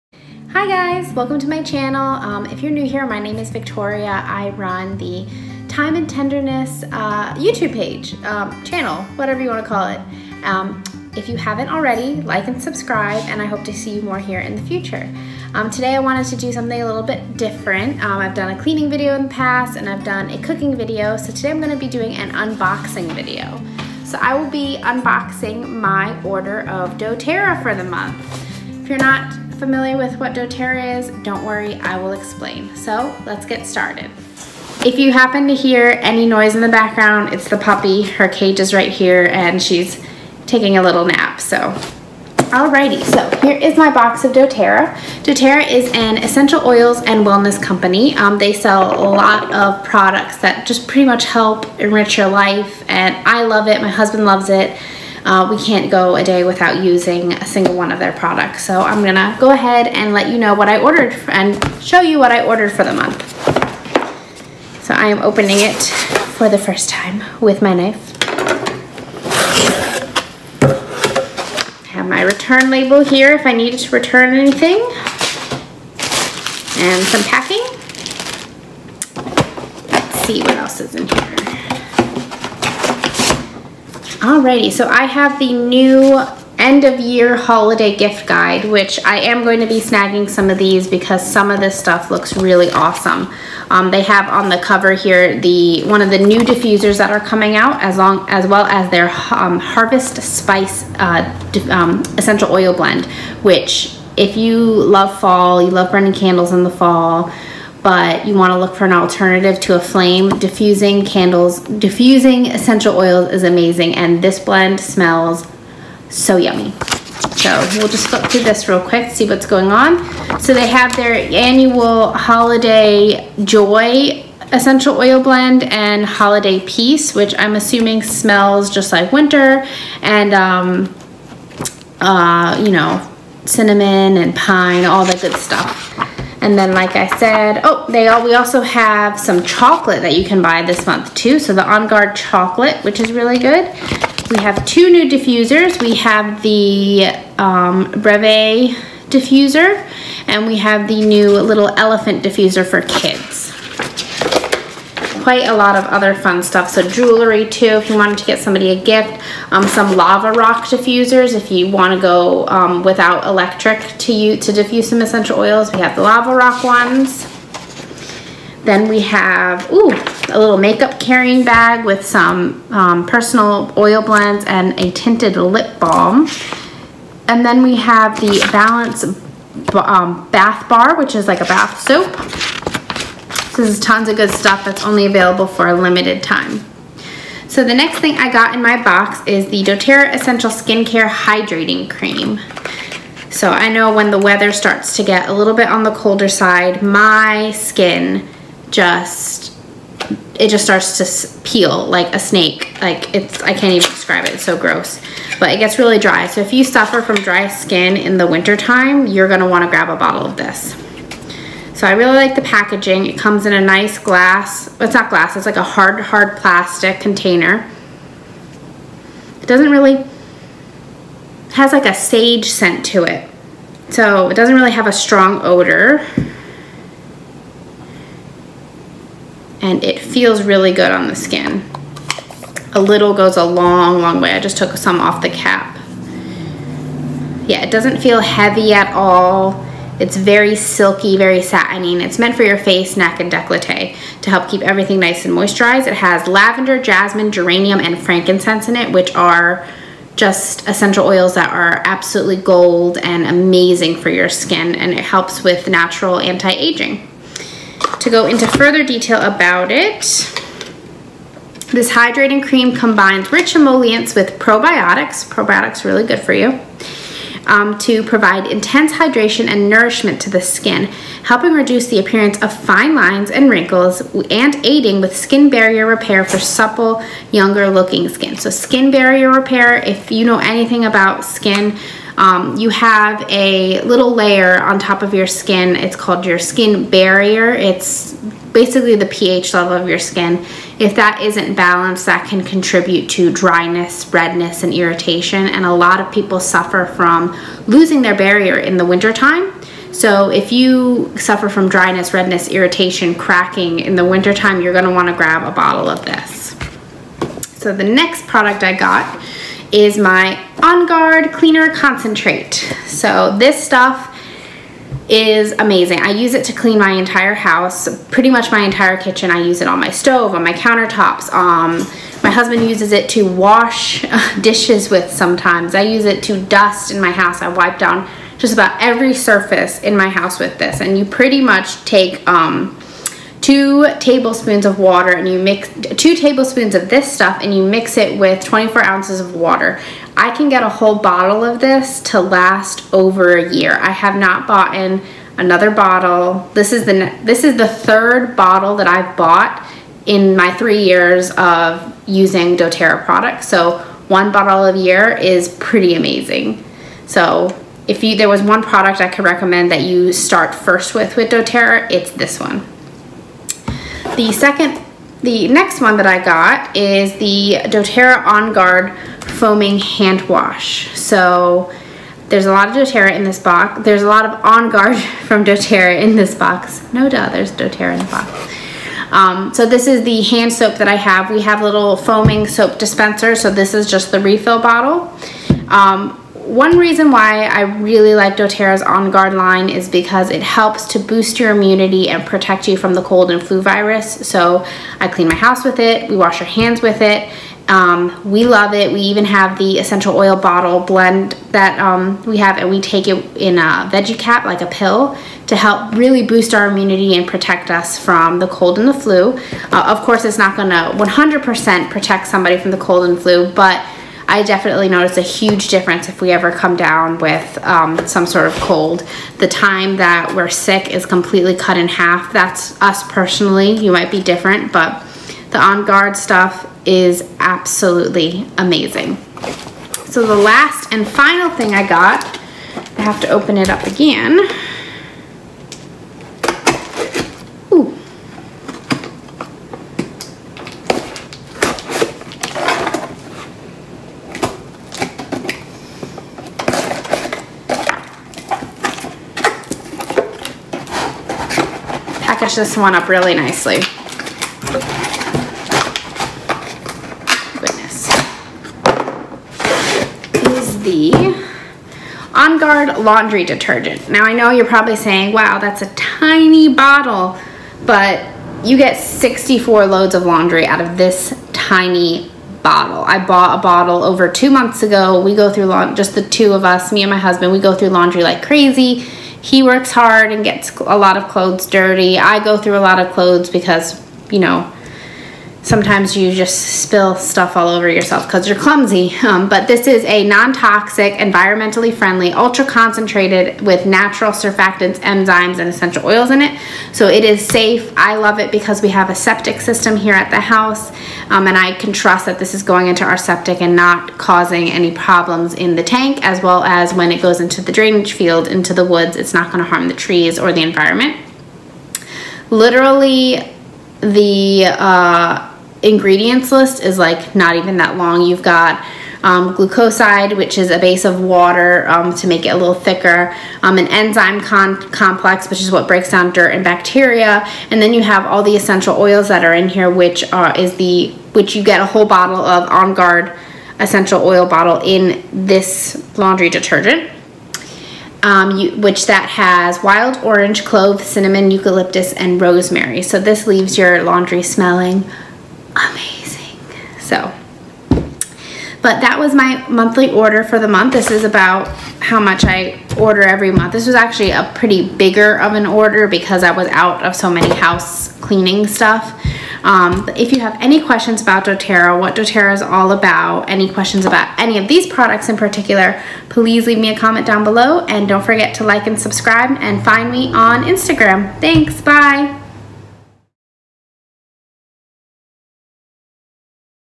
Unbox With Me! | Doterra | Home care
Please Excuse how awkward I am, I am still trying to get used to being in front of the camera!! Haha

I waned to take the time to share what I got this month from DoTERRA.

DoTERRA is an Essential Oils company that sells MUCH more than just oils. I use DoTERRA products everyday! I cook with them, I clean with them, and they are even part of my every day beauty routine. DoTERRA is such an important part of mine and my husband's life! I just wanted to share a teeny teeny bit of how amazing DoTERRA is with you!